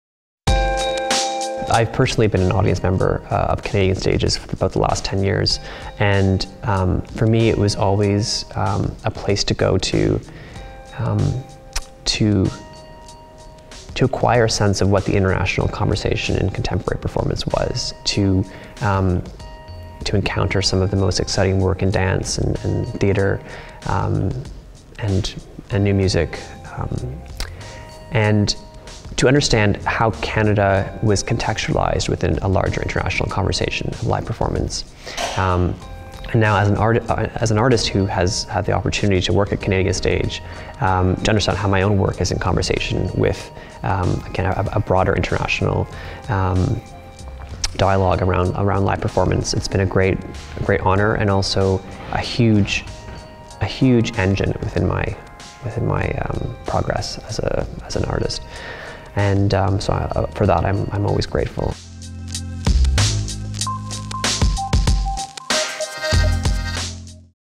Jordan Tannahill on 30 seasons of Canadian Stage
We sat down with writer/director/performer Jordan Tannahill, currently in rehearsal for his latest work, Declarations (on stage Jan 2018), to talk about his memories of Canadian Stage as a young patron and emerging artist.